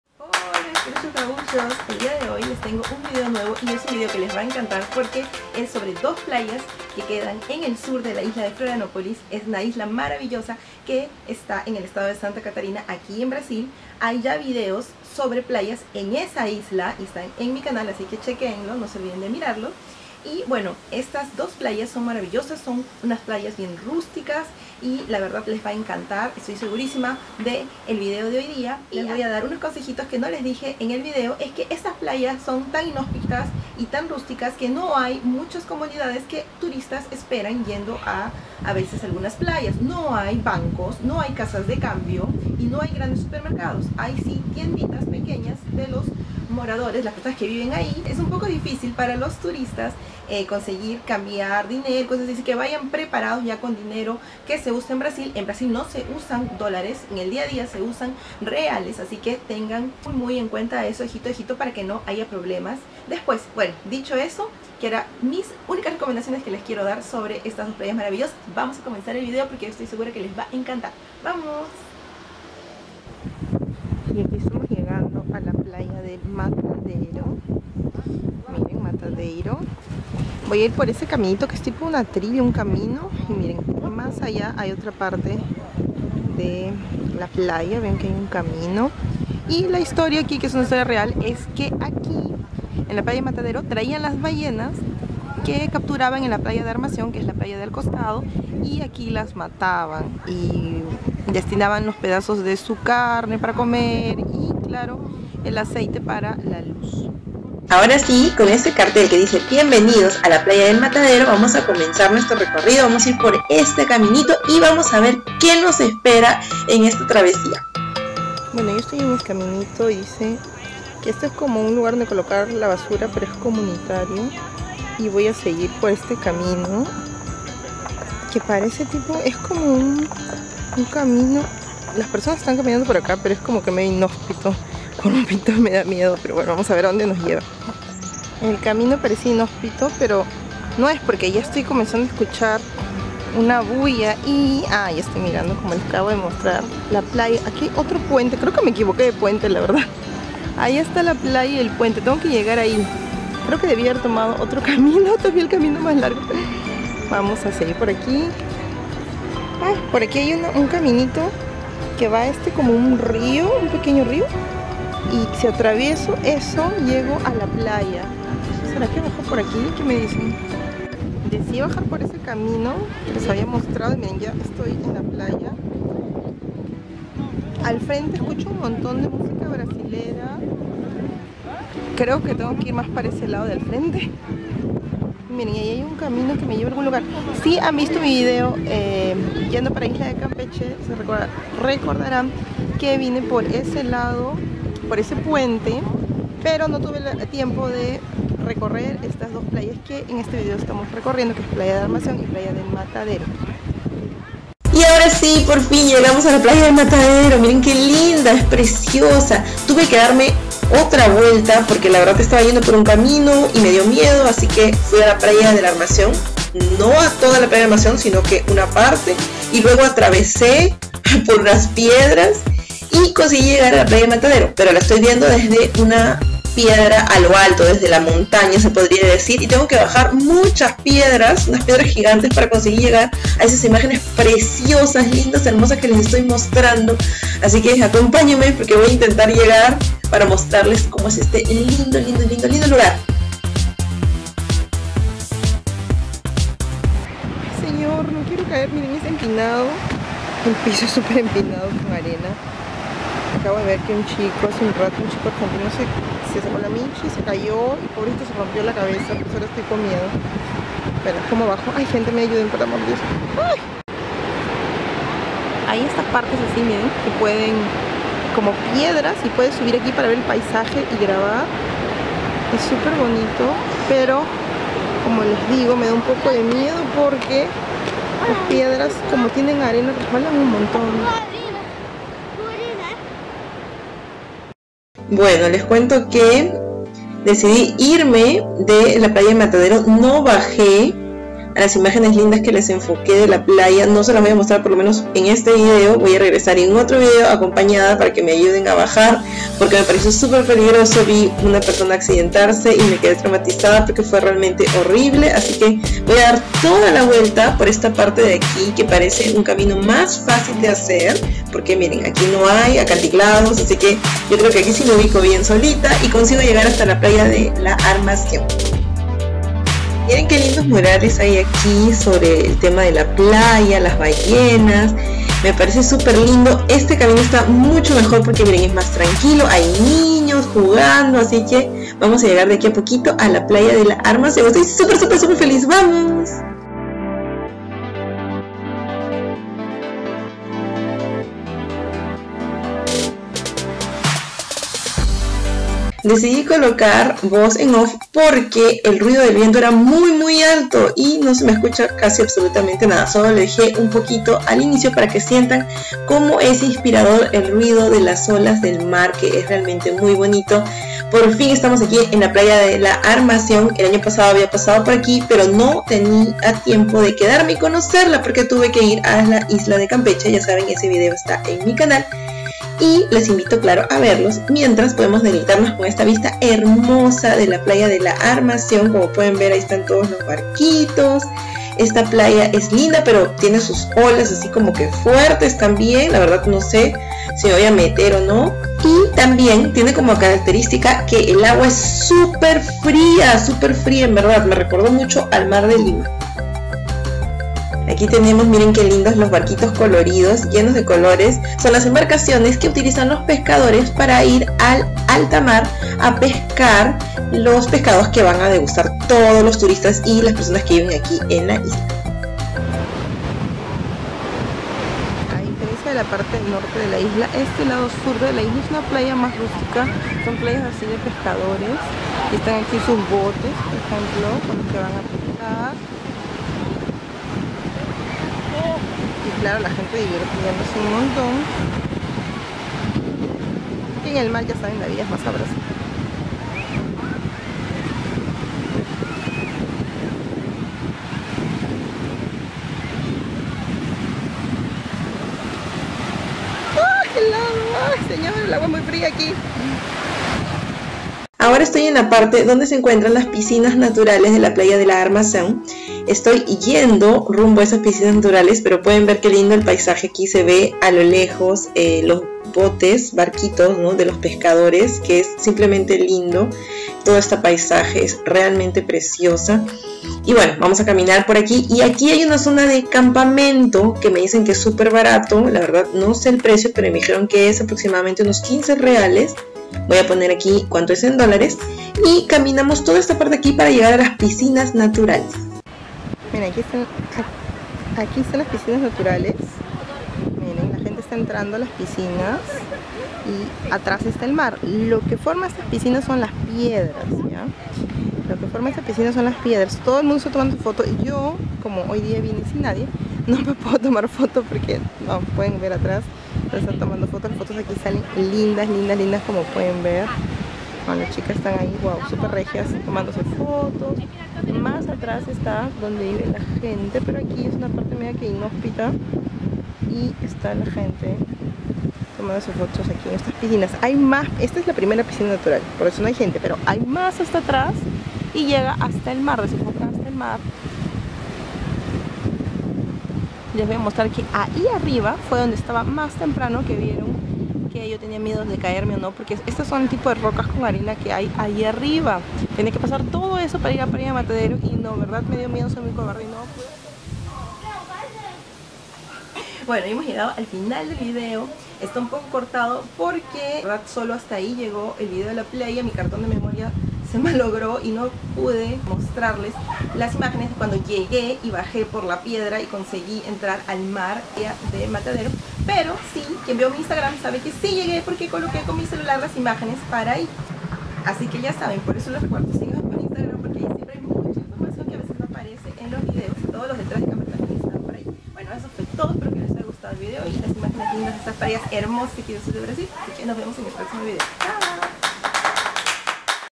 PLAYAS BRASILERAS - ARMAÇÃO E MATADEIRO/ PISCINAS NATURALES en FLORIANÓPOLIS - BRASIL
Hola Peruchos Gaúchos! les agradezco por acompañarme en más un video en mi canal, esta vez les voy a mostar dos playas lindas y paradisiacas del sur de la isla de florianopolis ( estado de santa catarina) sur de Brasil! tbn las piscinas naturales de la playa de armaçao, lescuento un poco la historia de estas dos playas : ARMACÇAO E MATADEIRO porque de esos nombres y como son actualmente ;) . Dejenme un like al finalizar el video y claro comentarios para saber si les gusto este video.